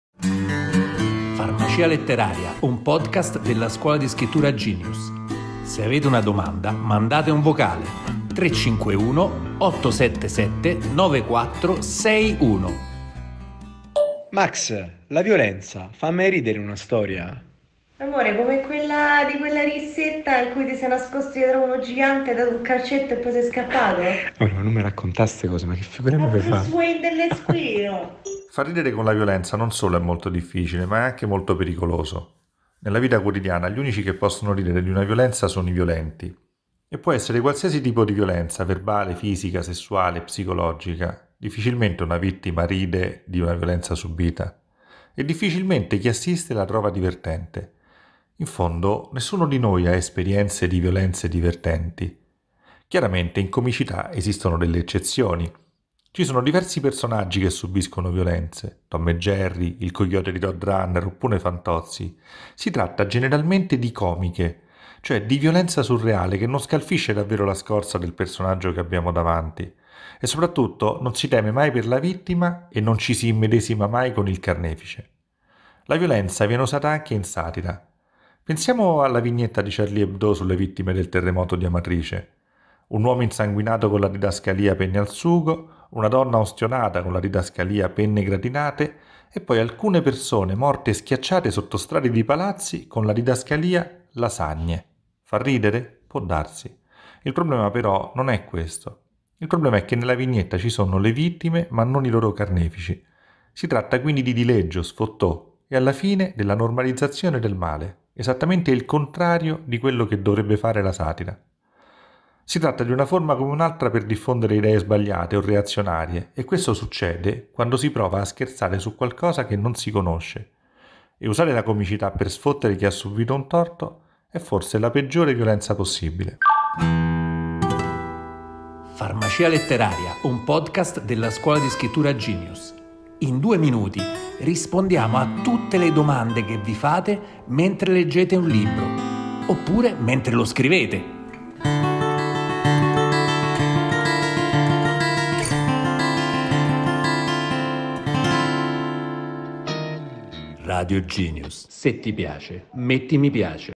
193 – VIOLENZA E COMICITÀ [scrittura creativa]
La violenza fa mai ridere in una storia?
Risponde Massimiliano Ciarrocca, autore del libro "Pronto Francè" (Fazi editore), ha collaborato con «Liberoveleno» e ha scritto lo spettacolo teatrale "Buon Natale, la trilogia del livore". Insegna in diversi laboratori creativi.